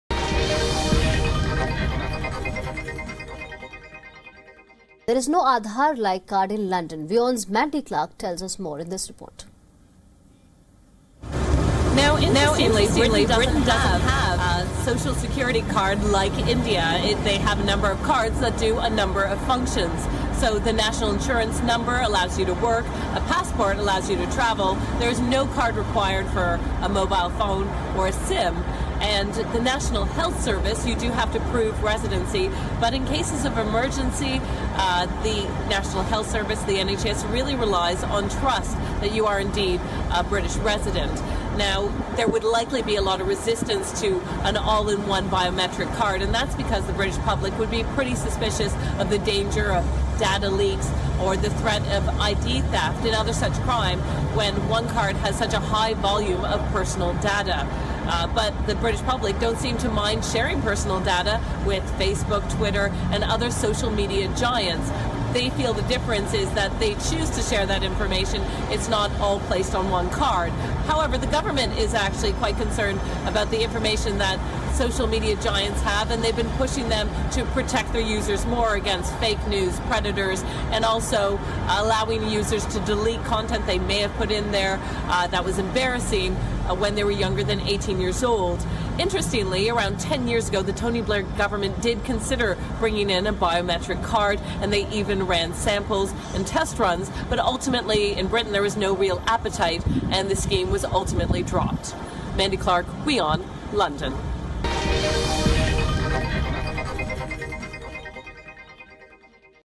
No bio metric card in the UK
No social security cards in the UK. Watch the full report to know more.